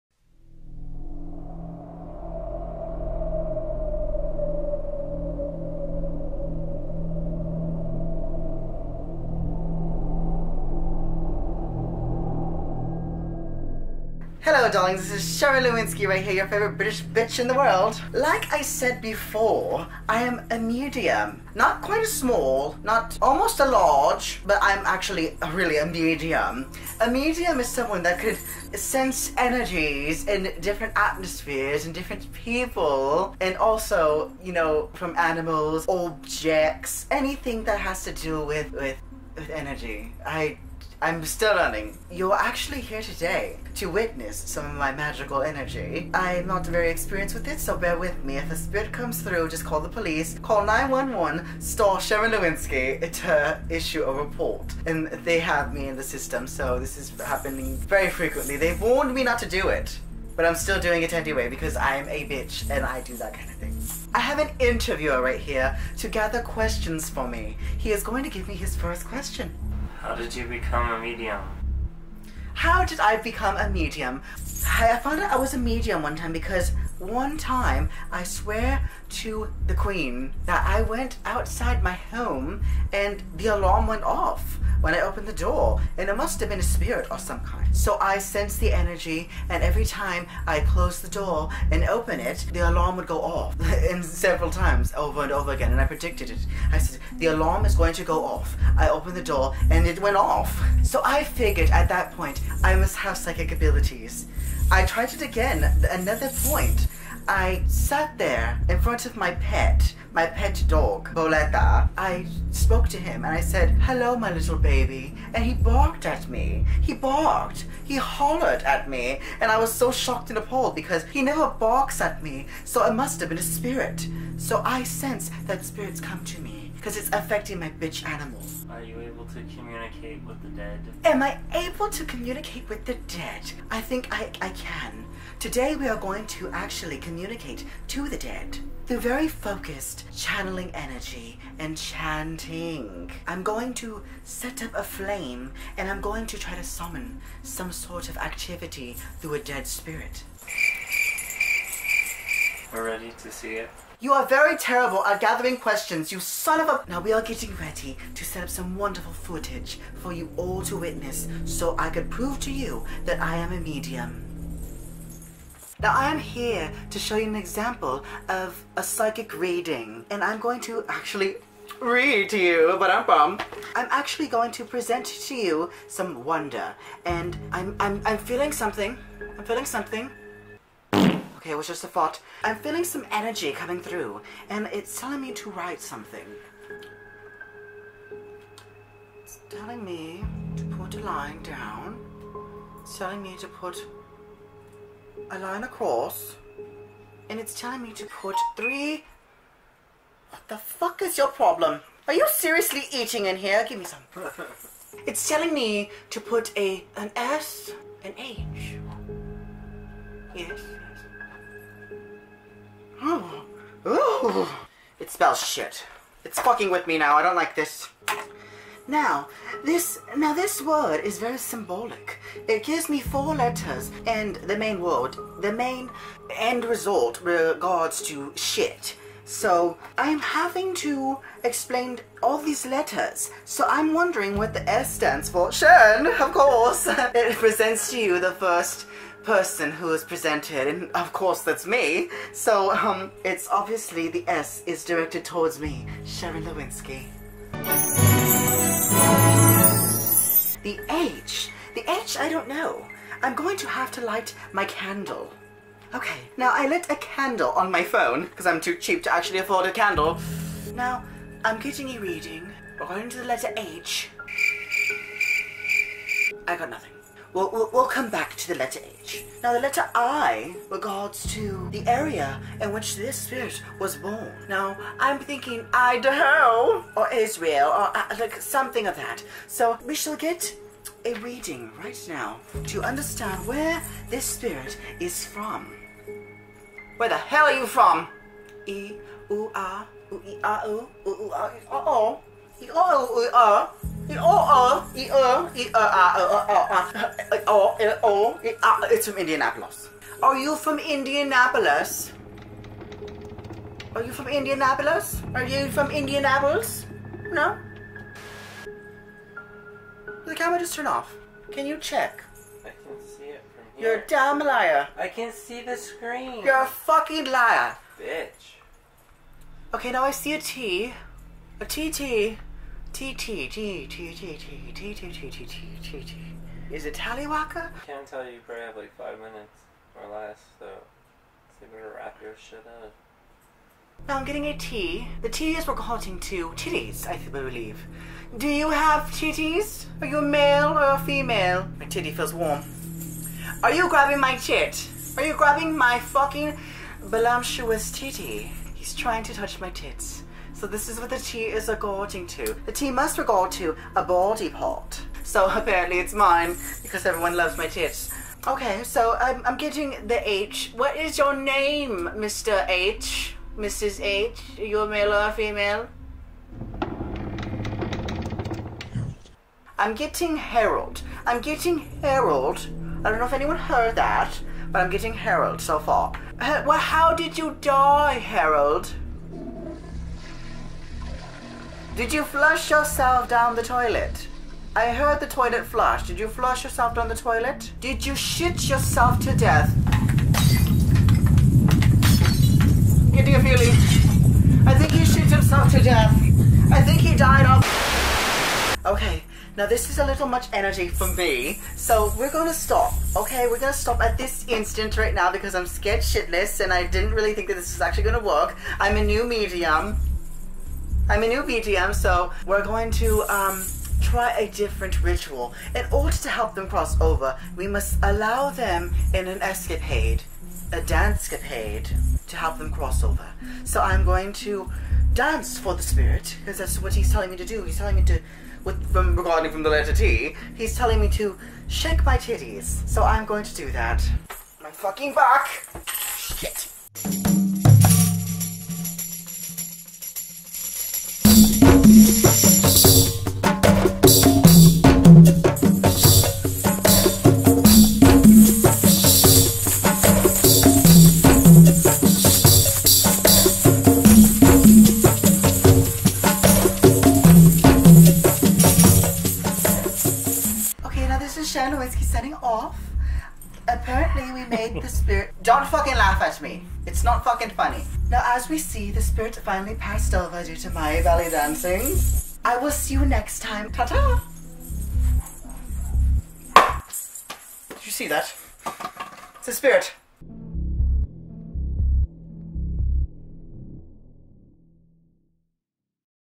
Sharron Looinsky Psychic Reading
Sharron is now a medium. join her in this video as she experiments with her gift.